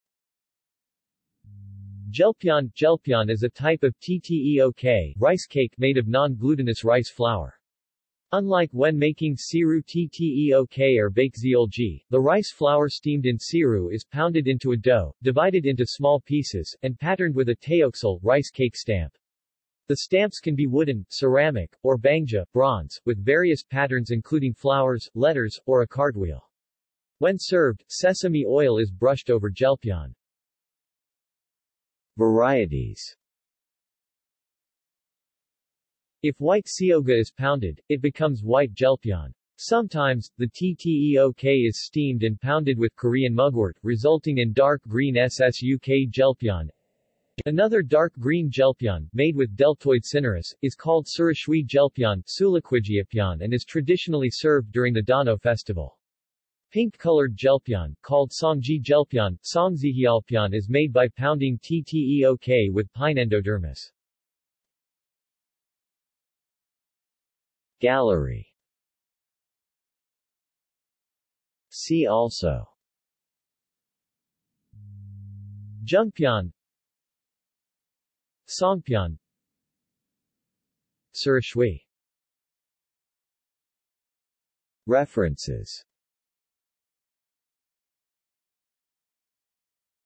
Jeolpyeon | Wikipedia audio article
This is an audio version of the Wikipedia Article:
Jeolpyeon

SUMMARY
Jeolpyeon (절편) is a type of tteok (rice cake) made of non-glutinous rice flour. Unlike when making siru-tteok or baekseolgi, the rice flour steamed in siru is pounded into a dough, divided into small pieces, and patterned with a tteoksal (rice cake stamp). The stamps can be wooden, ceramic, or bangjja (bronze), with various patterns including flowers, letters, or a cartwheel. When served, sesame oil is brushed over jeolpyeon.